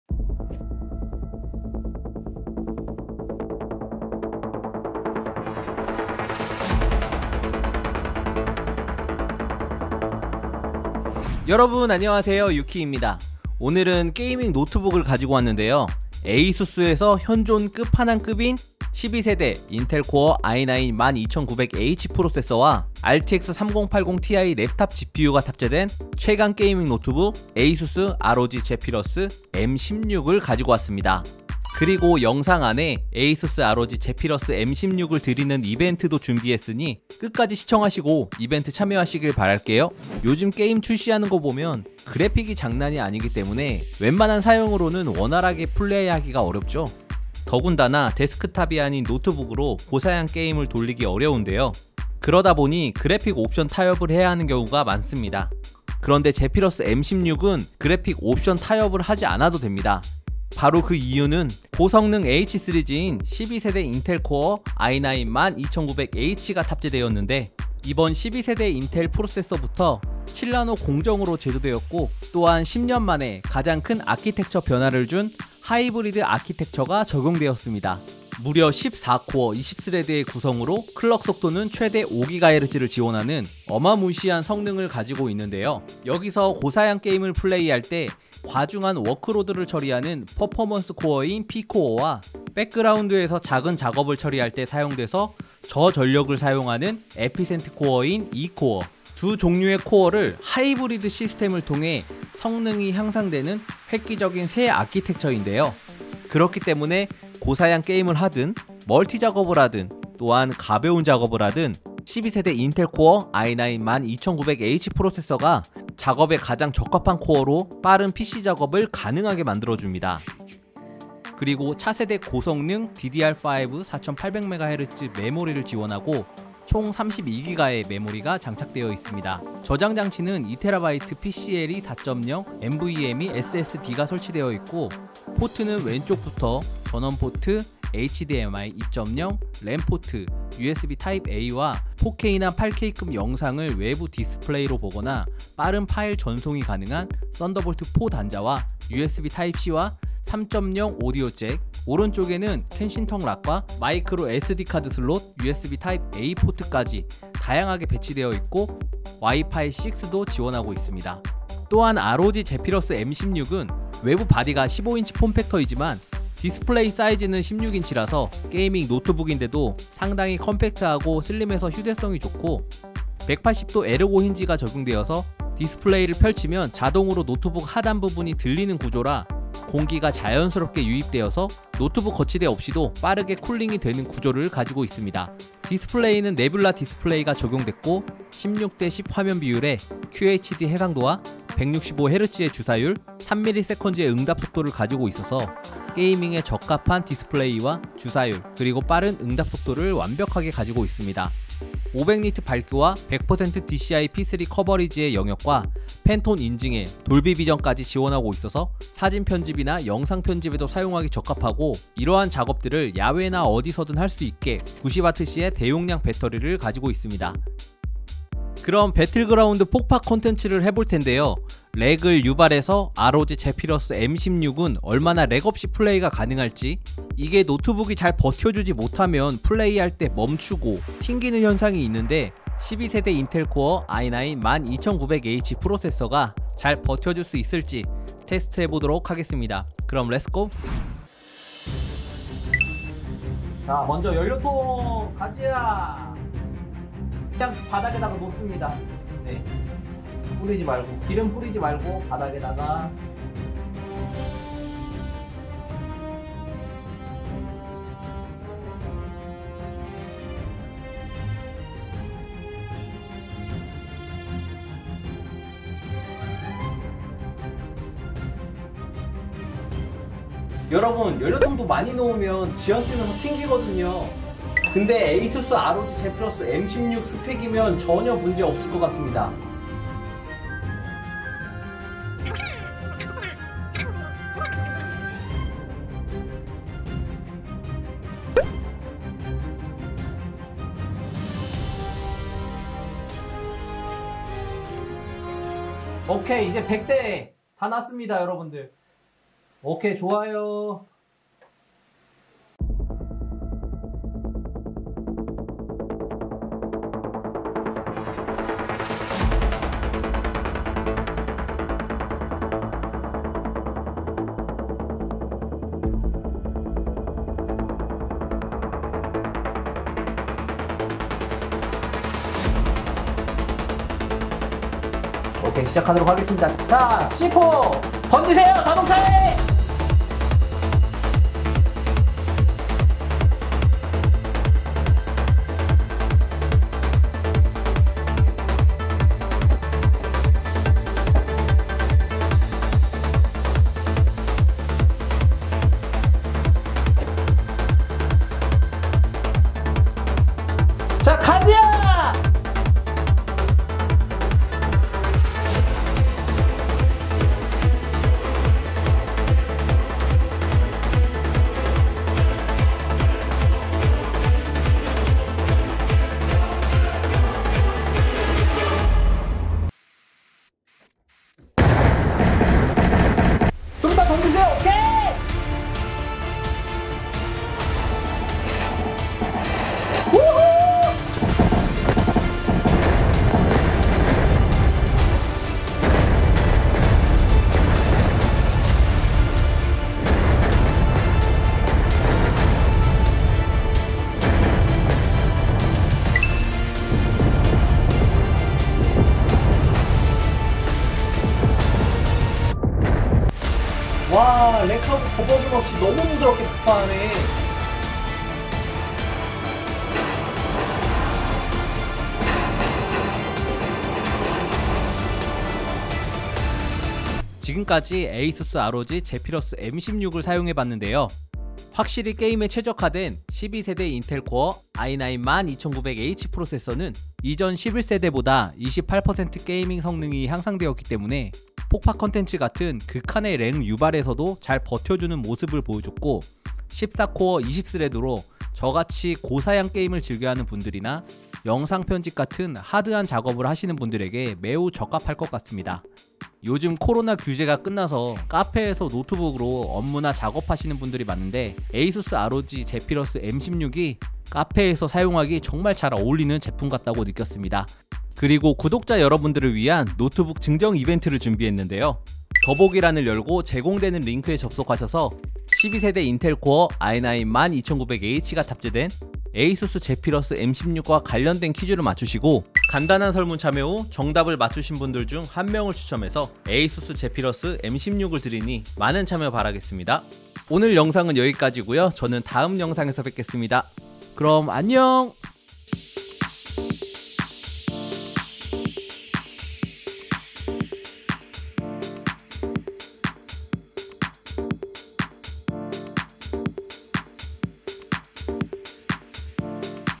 대박!!12 세대 인텔® 코어™ i9 프로세서가 탑재된 게이머들을 위한 끝판왕 노트북 ASUS ROG Zephyrus M16 리뷰
📍12세대 인텔 i9-12900H와RTX 3080Ti가 들어간 게이머들을 위한 끝판왕 게이밍 노트북 ASUS ROG Zephyrus M16을 폭파 컨텐츠로 성능 테스트를 해봤습니다!!